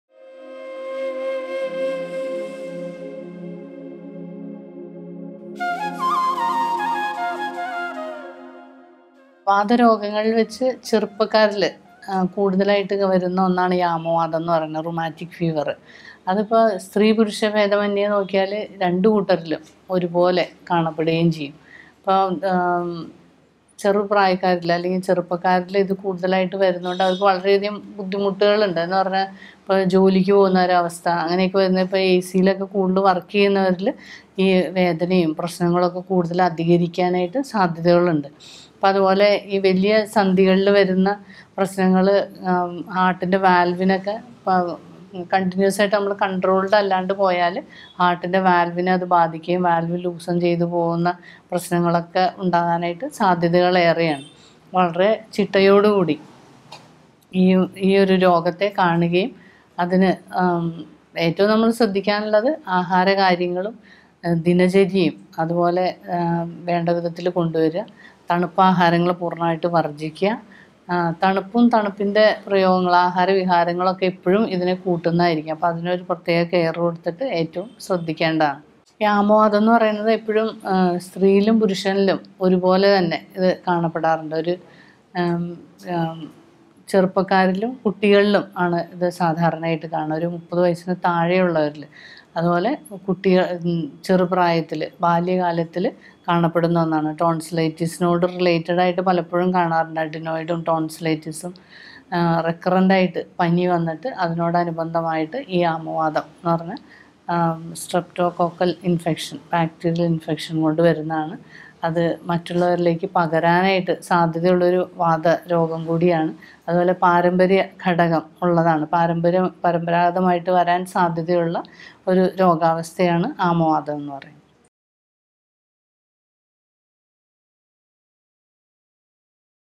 Rheumatoid Arthritis in Young People | Causes & Treatment | Manorama Online
Rheumatoid Arthritis in Young People; Causes of This

Rheumatoid Arthritis is never more an issue of the old. This sickness is found now also among the youth. So Dr. Rosemary Wilson, Chief Physician, Kandamkulathy Ayurvedic Hospital, Thrissur speaks how far young people are prone to Rheumatoid Arthritis.

Rheumatism Syndromes - Pain in Your Toe; Why you should not ignore the pain in your toe?

Back Pain Vs Long Sitting Hours; How People Who Sit for Long Hours Risk Back Pain

Interview with Dr. Rosemary Wilson, Chief Physician, Kandamkulathy Ayurvedic Hospital, Thrissur
Producer: Sam David. P
Assistant Producer: Rekha. M. R
Camera : Tony Issac
Editor: Vin Iris
Head, Content Production: Santhosh George Jacob